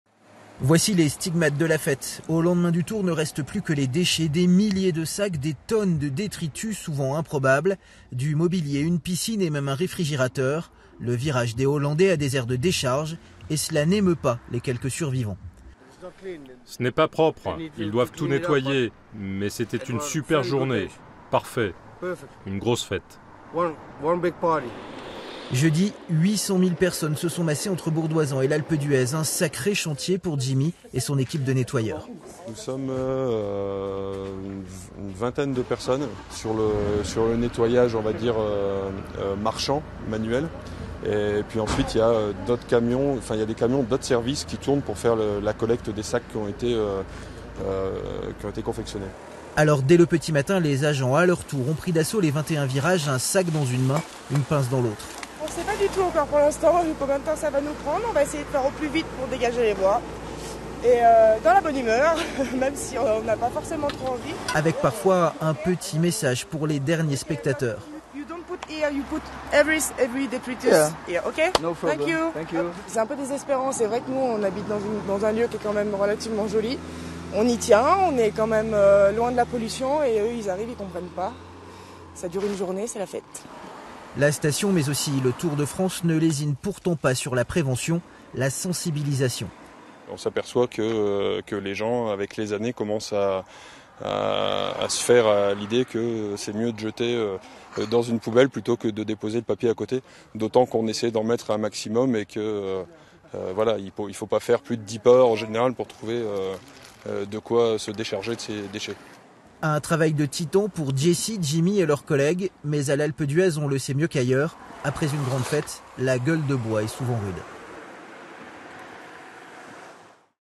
Canettes, détritus, frigo abandonné... le Tour de France a laissé des traces à l'Alpe d'Huez
Un reportage de Jordan Guéant et Grégory Lespinasse

Retrouvez-nous sur :
Notre site web